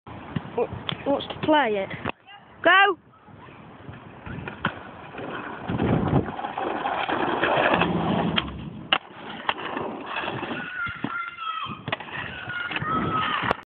i ollie off a kicker
i tried to 180 it but landed on my head .owwww